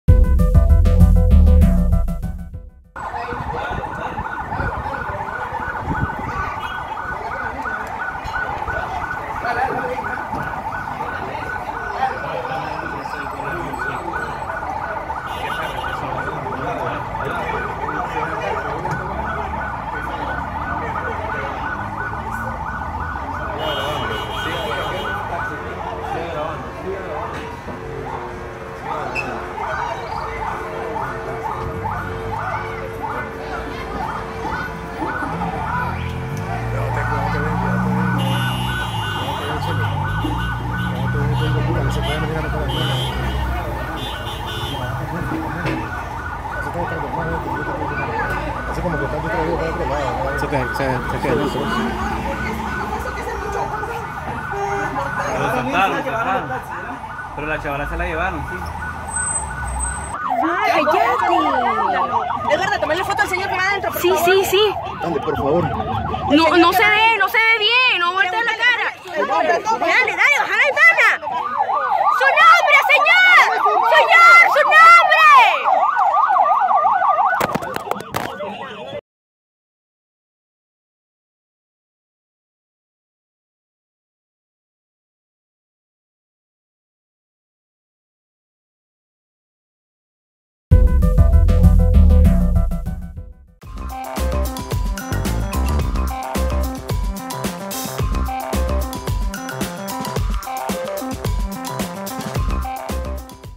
Así detiene la Policía de Nicaragua a los manifestantes
crisis nicaragua • policia de nicaragua • paramilitares • presos politicos • nicaragua • tv • video • noticias • noticiero • canal • medios

La Unidad Nacional Azul y Blanco reportó al menos 68 detenidos en Nicaragua este miércoles durante una protesta contra el Gobierno, en la víspera del primer aniversario del estallido social del 18 de abril de 2018.

¡Síguenos!